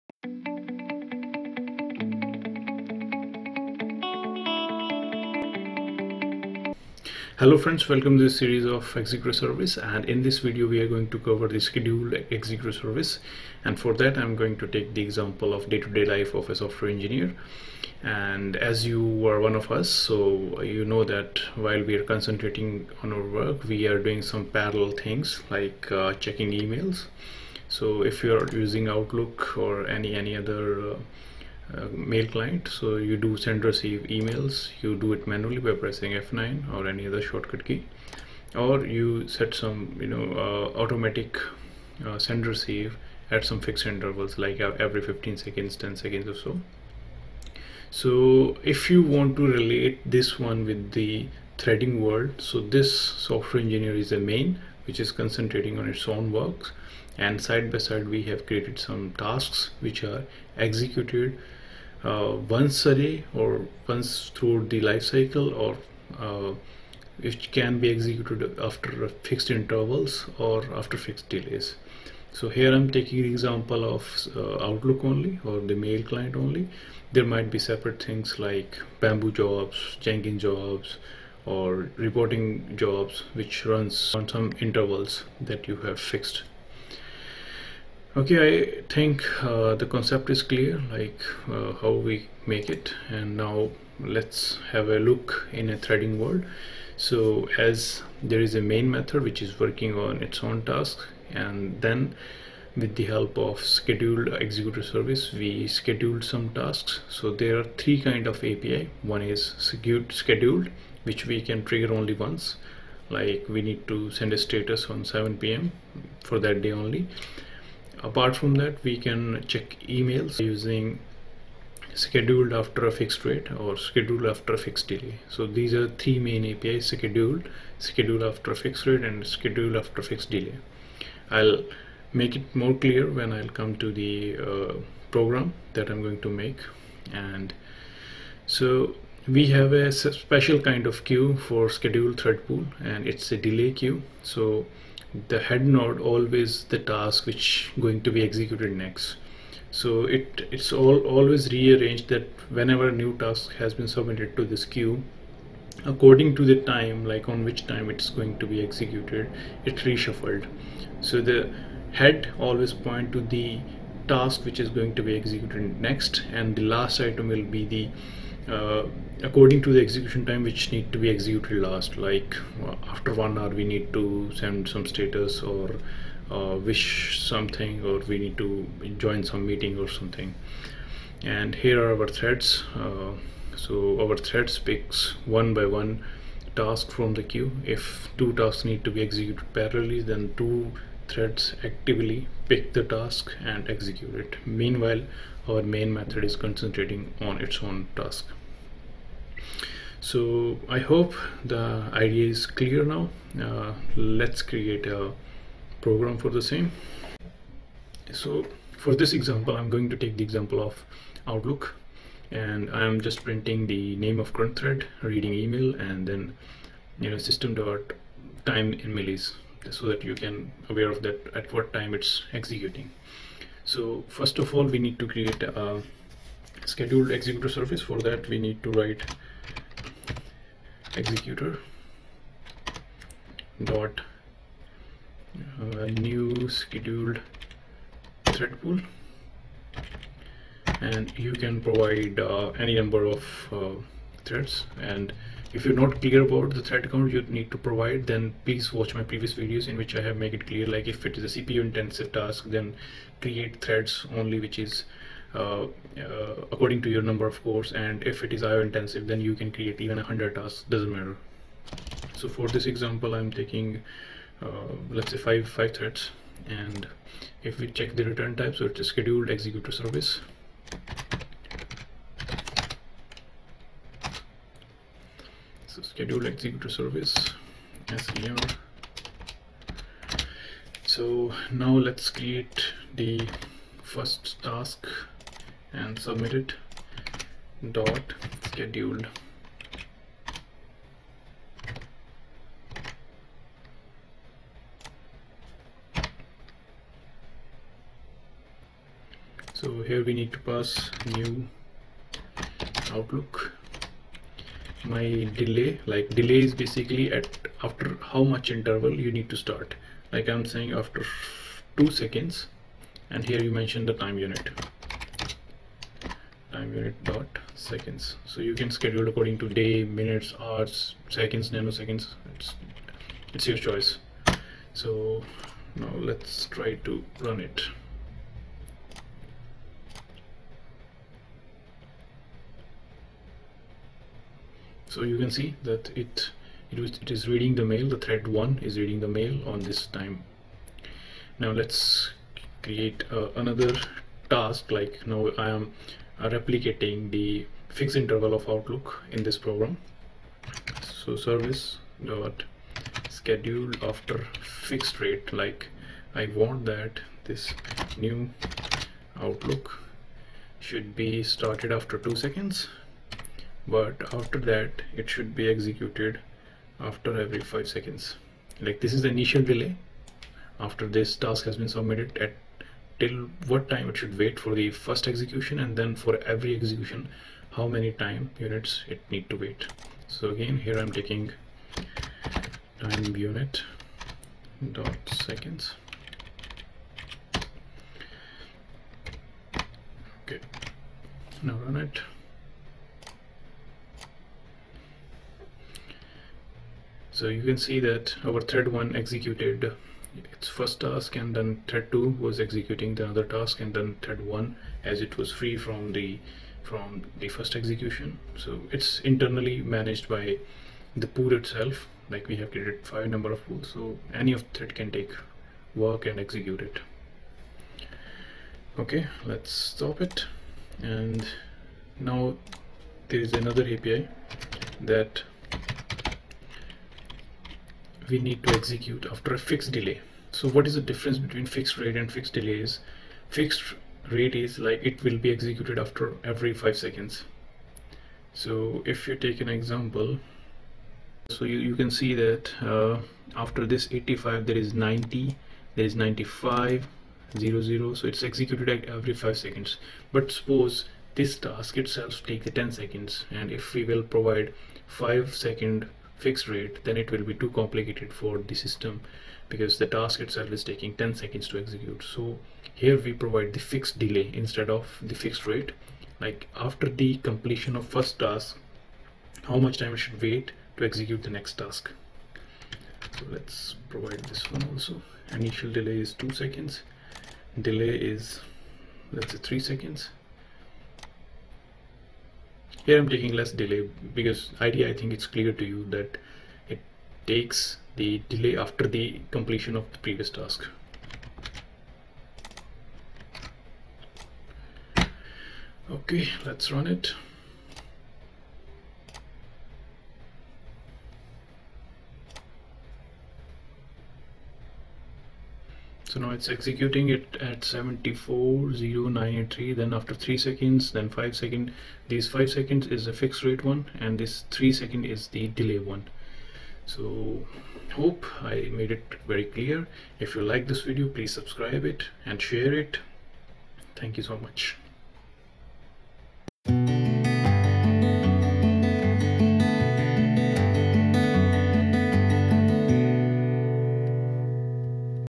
Executor Service 04: Scheduled Thread Pool
Internal Implementation of  Scheduled Thread Pool.
 How it works. Way to schedule your Runnable. How to set tasks after after fixed delay and at fixed rate.

These Runnable/Callable are organized in Delay Queue and Threads Pick and execute tasks from delay queue on the basis of frequency defined.